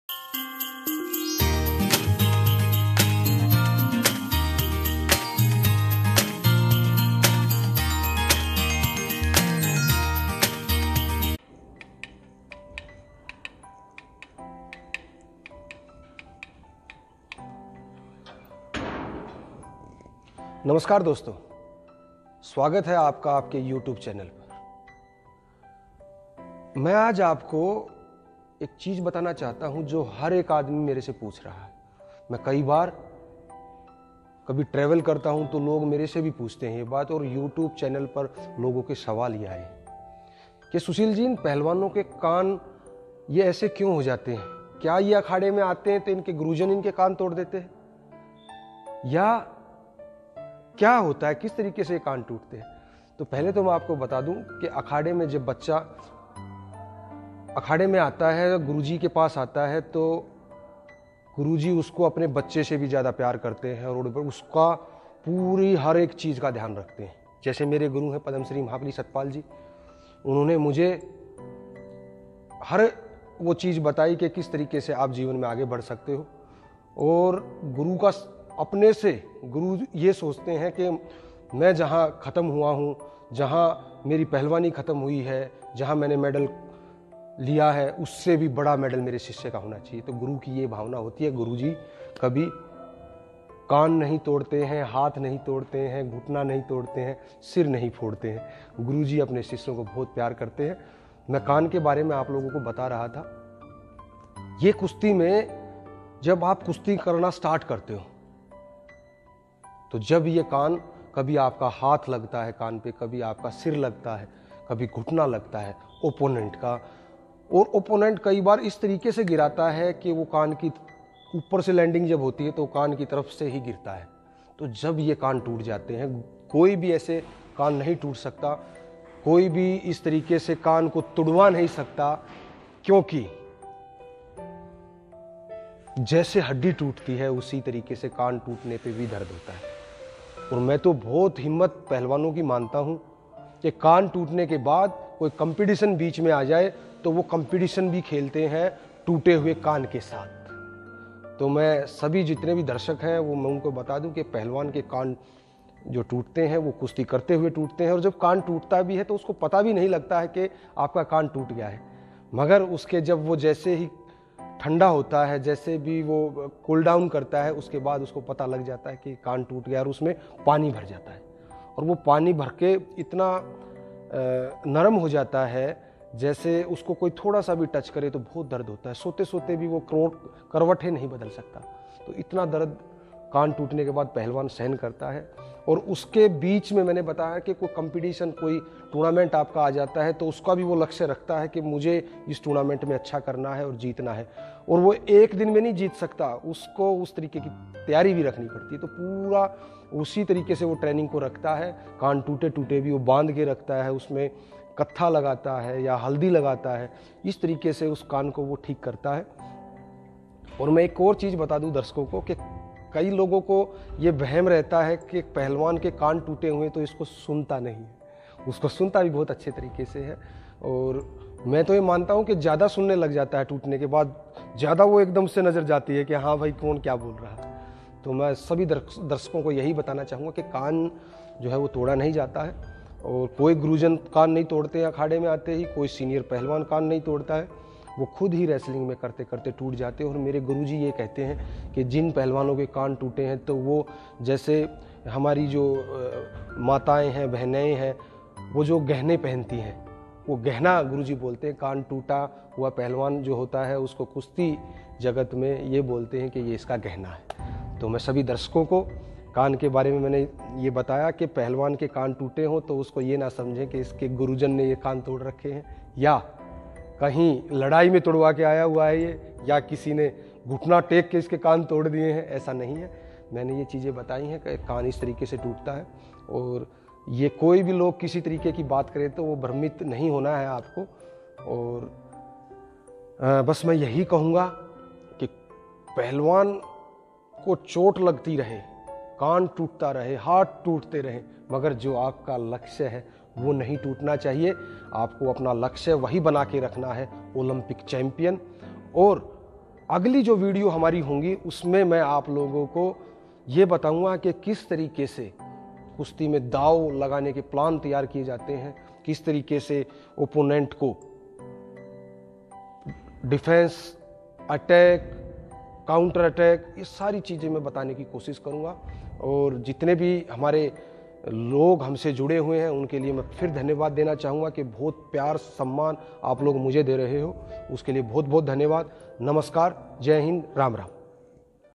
Why Grapplers Have Disfigured Ears? Wrestler Sushil Kumar
Watch and learn about the myths of wrestlers' disfigured ears.